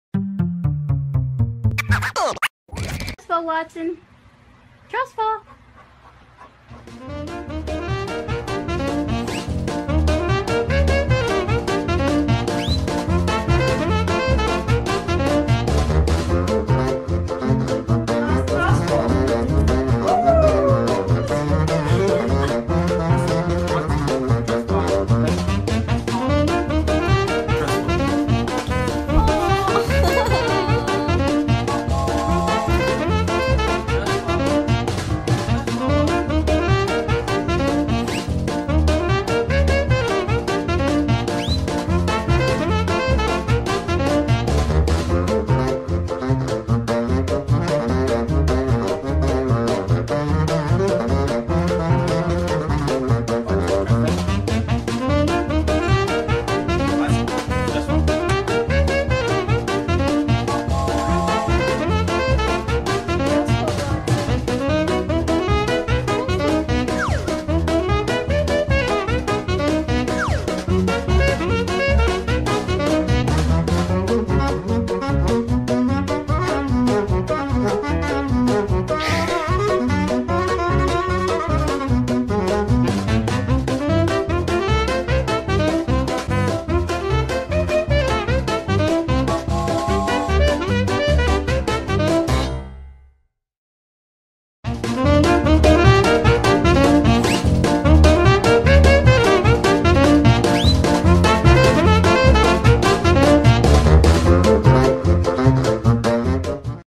Dog Trust Fall
Dog trust fall – it’s an incredibly cool dog trick that you absolutely have to see.
In this dog trust fall compilation you’ll see super smart dogs trust fall with owners  and even one dog trust fall into teddy bear!
If you watch this dog trust fall video till the end, you’ll find a little surprise… CAT trust fall! Now that’s just something everyone should see with their own eyes!
I hope you’ll like this dog trust fall compilation. If so, please give a like:)